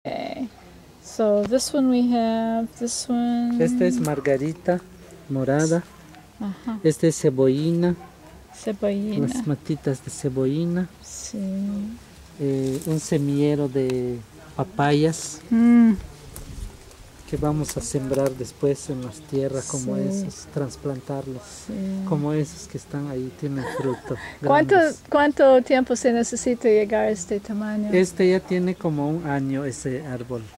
Joaquin's "solar" (Maya home garden) - some of the plants
A Maya home garden ("solar") in Felipe Carrillo Puerto, Q. Roo